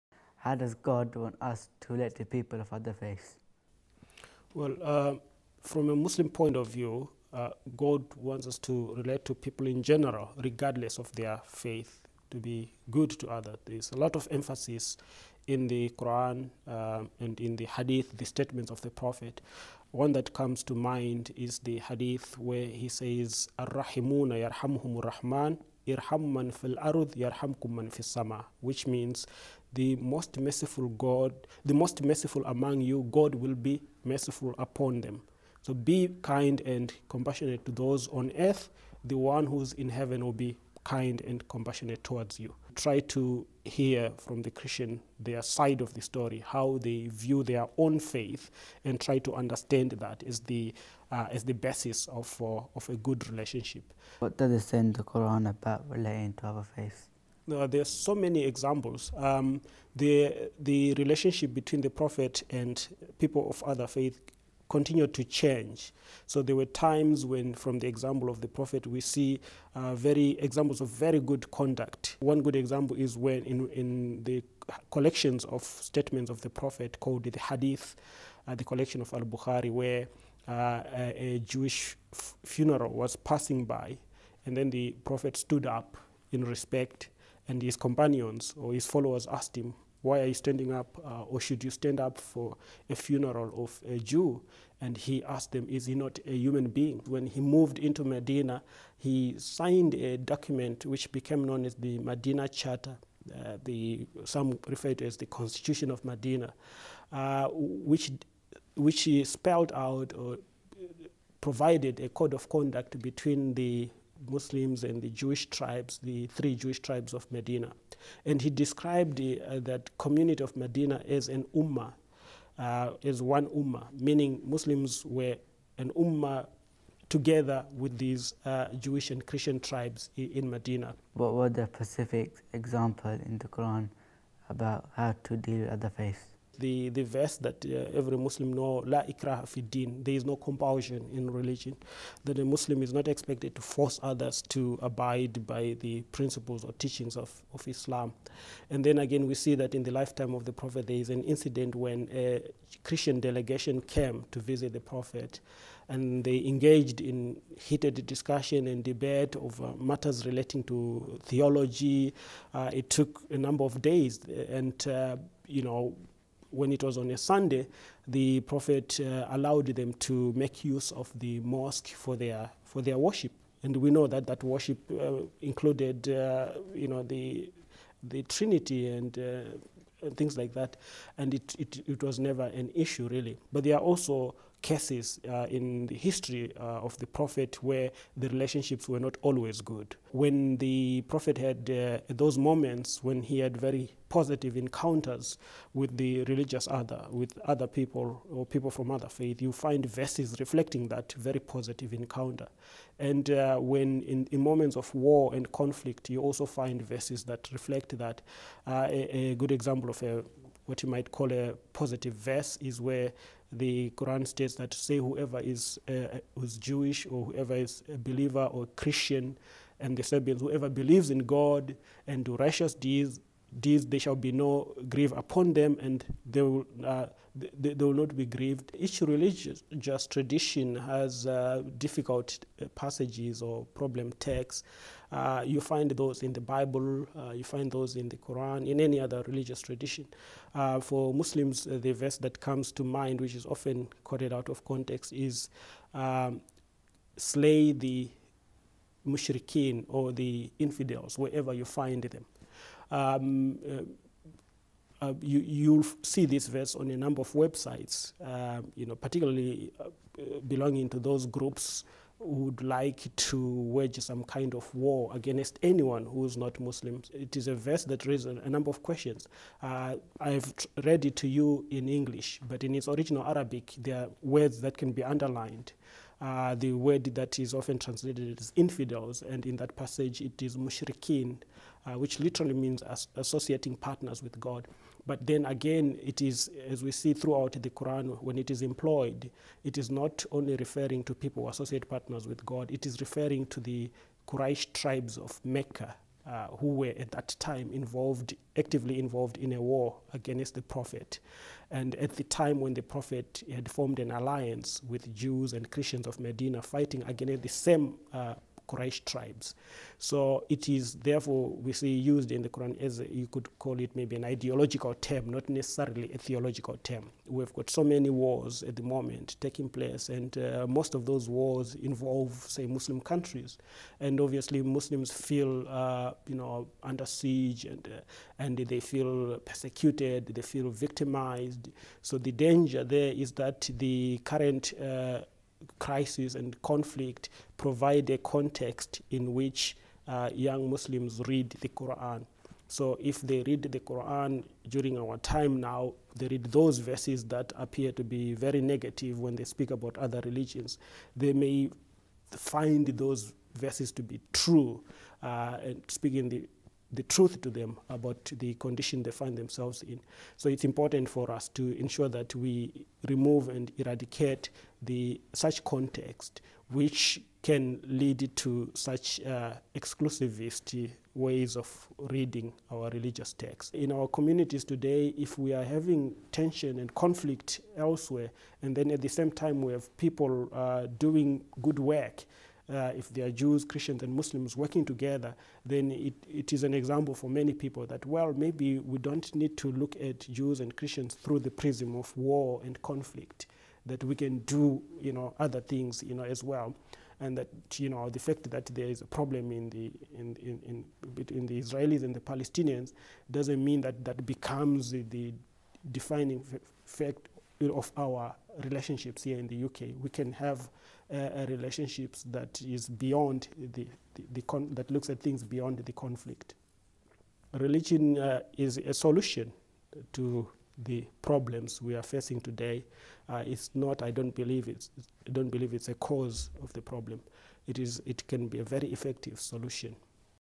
Shaykh Michael Mumisa
Shaykh Micheal Mumisa is a Muslim scholar who grew up in South Africa. He tells us how Muslims should relate to Christians and Jews based on the Prophet's example and what the Qu'ran says about the subject. We're sure you'll find what he has to say is very interesting! :)